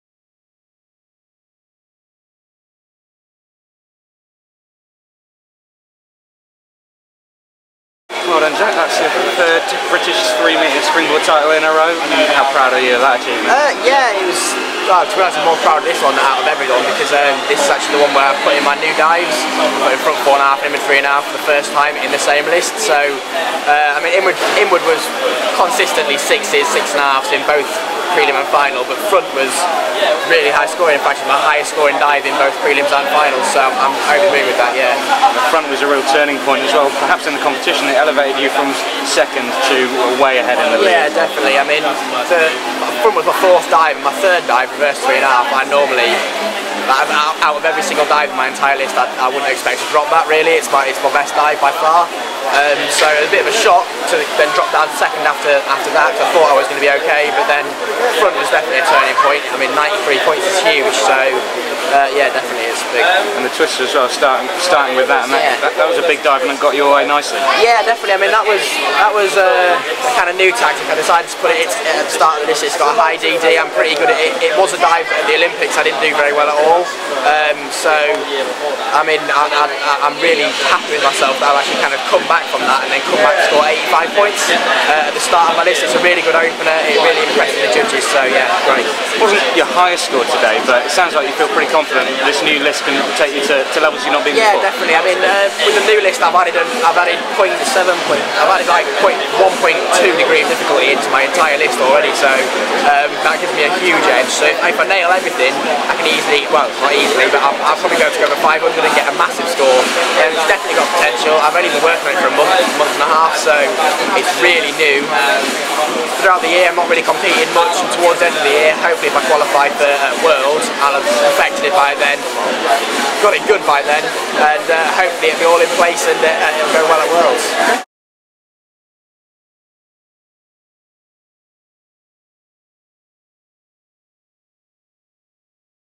Jack Laugher wins 3m Springboard gold at the 2013 British Champs
Jack Laugher defends his title to secure his third consecutive national 3m Springboard title at the 2013 British Gas Diving Championships in Plymouth.